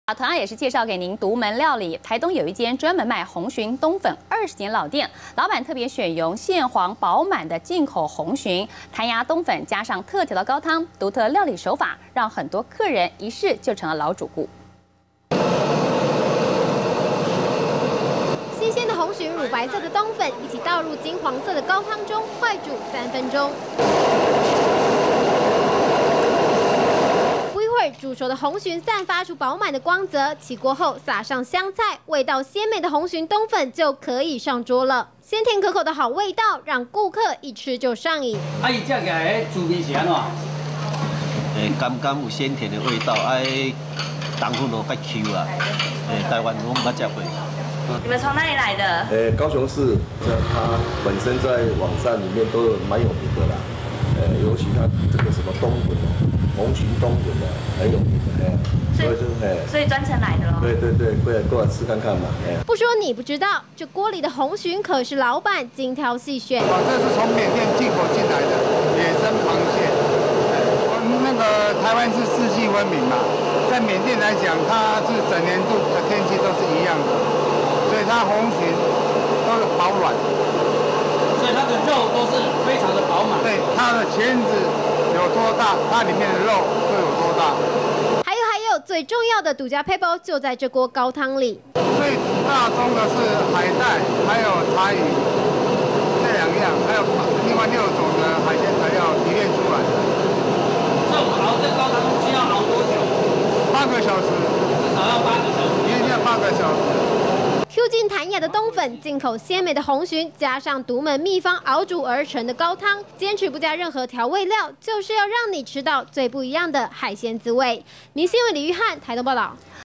紅蟳冬粉鍋 緬甸紅蟳蟹黃香－民視新聞
【民視即時新聞】台東有一間專賣紅蟳冬粉的20年老店，老闆特別選用了蟹黃飽滿的進口紅蟳，Ｑ勁彈牙的冬粉，加上特調的高湯，獨特的料理手法，讓許多客人一試就成老主顧。